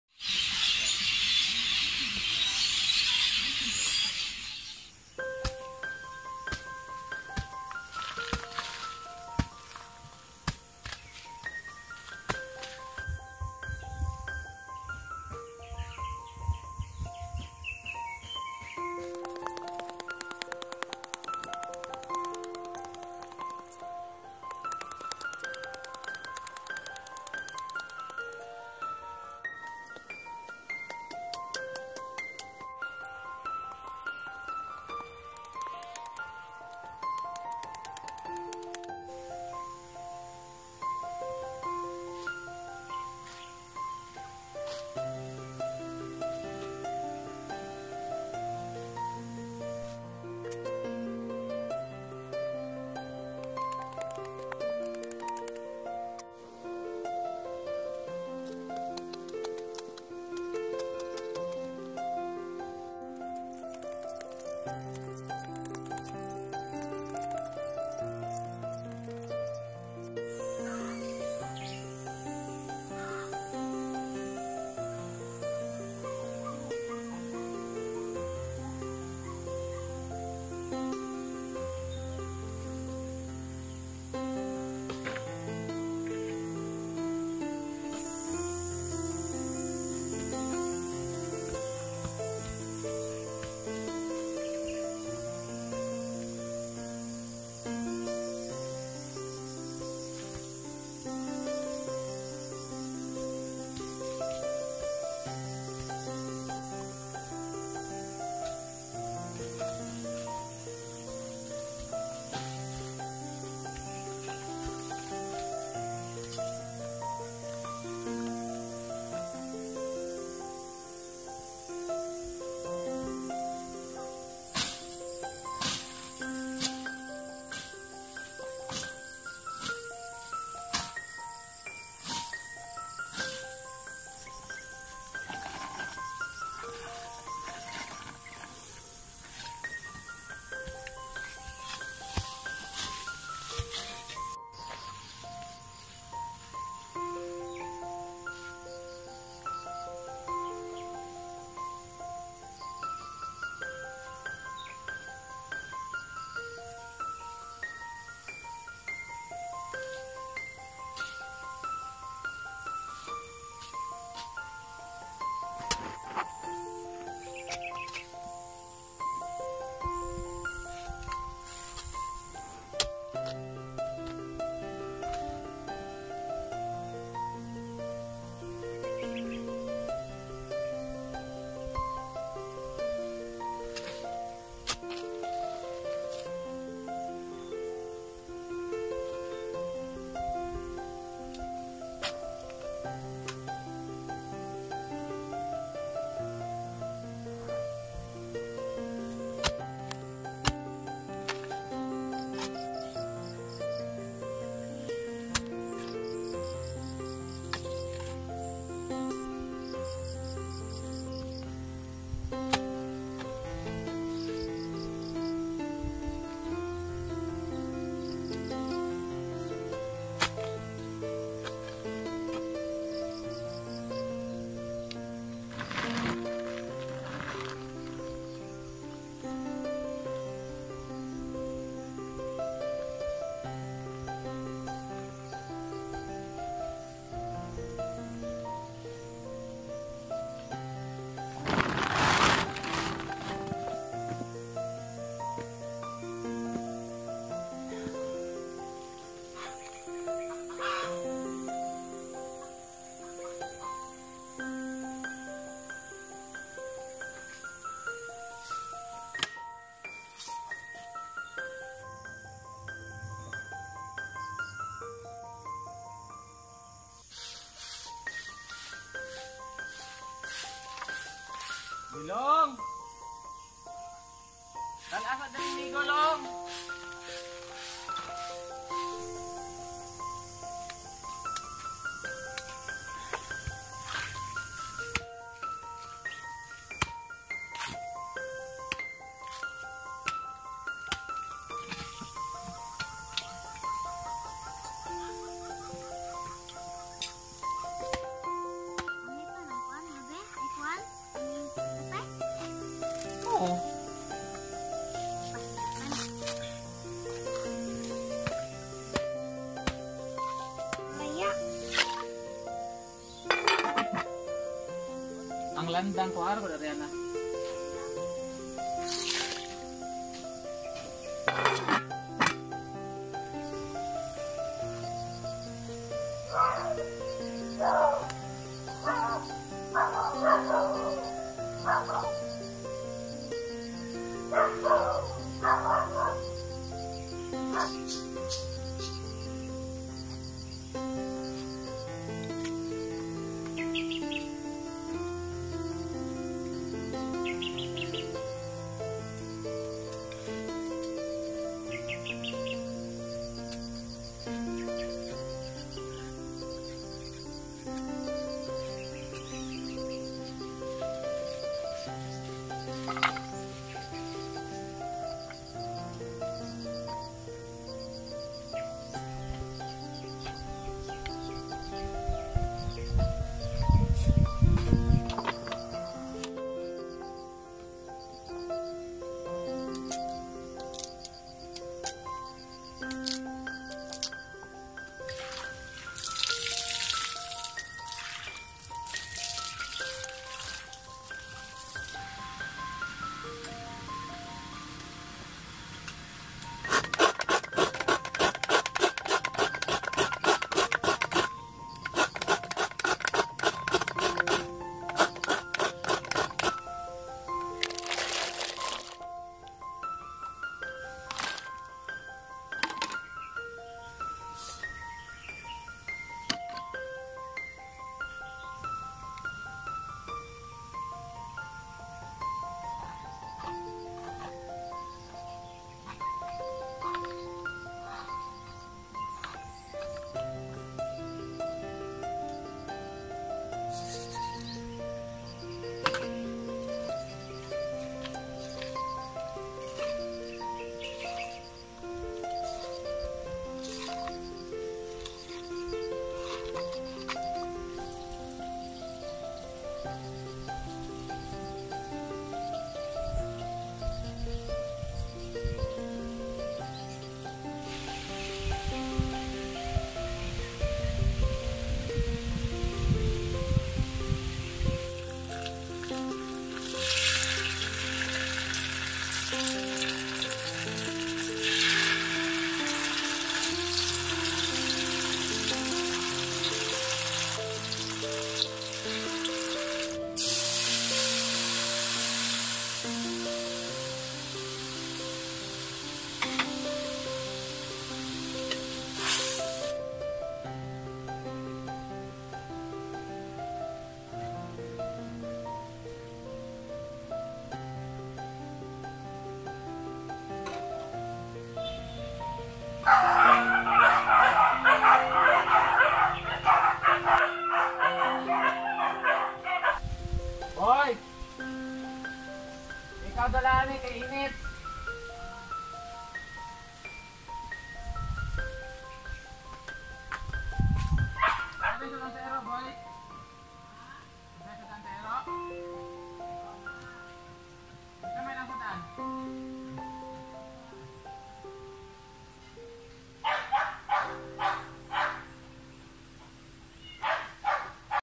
Life in the Mountain, Sweet Potato |Kuya Dhons Vlog
Hello friends,

Welcome back again here to my channel 💗
I made some delicacies of sweet potato I hope you all enjoy watching 💗